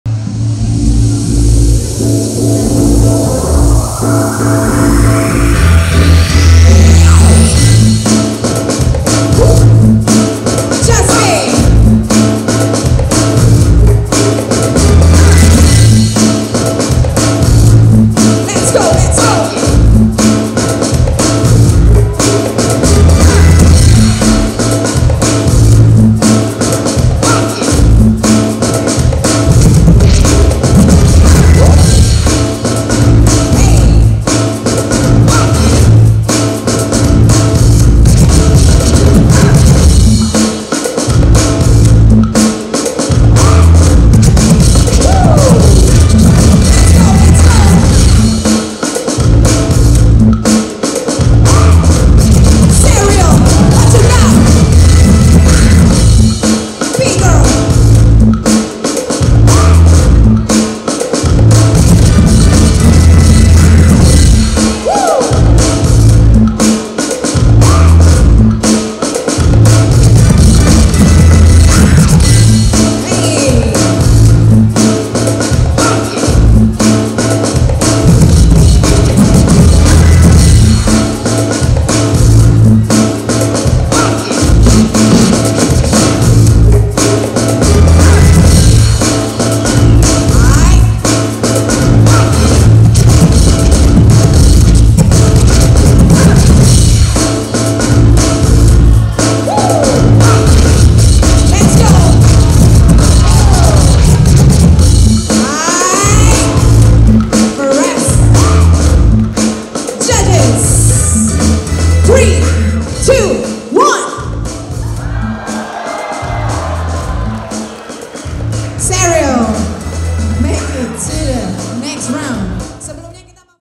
JAZBE vs SHERLY | BREAKIN TOP 8 | EDB EXPANSION 2023
Thank you for all the judges!!
Andreystylez
Shutdown
P-Lock
Marid
Wing
Gawblin
kreate
Mc Semmy Blank, Novie One, Gunz
Dj J-kribox & DJ Gandhi Fruitless
Organize by Rocket Crew Production
Video By Vibrance Studio